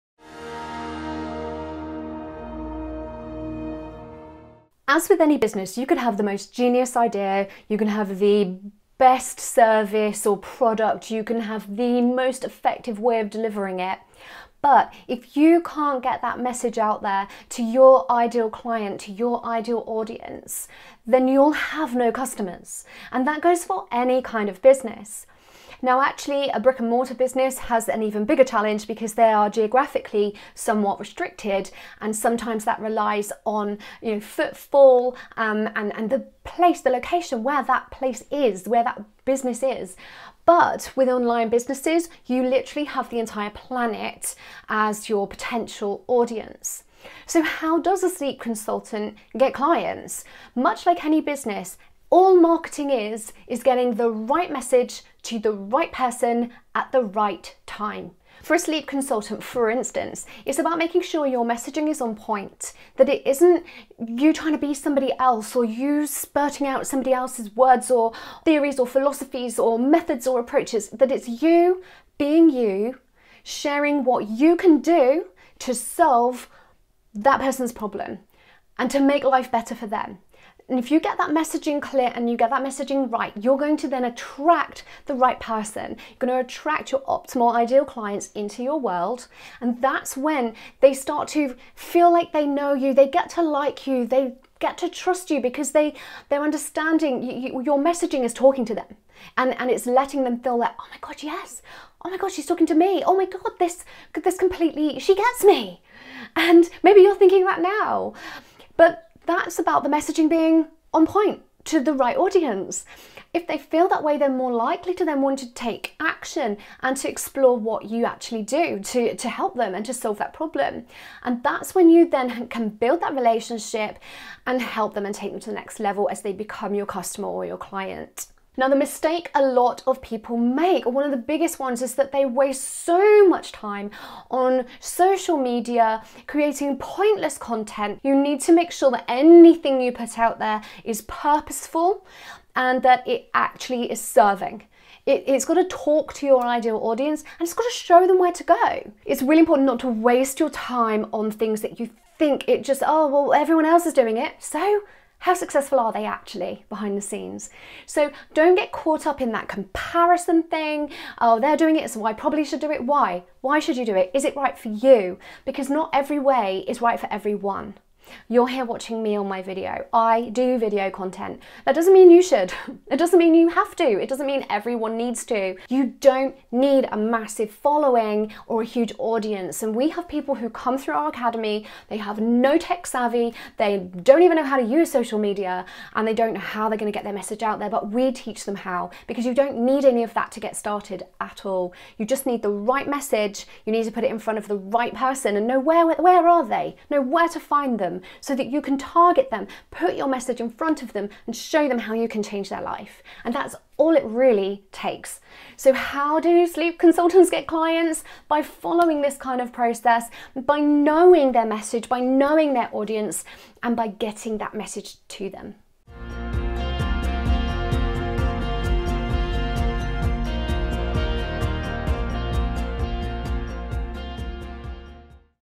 How To Get Clients As A Sleep Consultant? (Sleep Consultant Advice)
How do sleep consultants get clients?

In this video I'm going to teach you how to get clients as a sleep consultant.

Getting consulting clients is very hard or very easy depending on what you're doing to get them...

Watch this video to find out how you can attract sleep consulting clients and grow your consulting business as a result!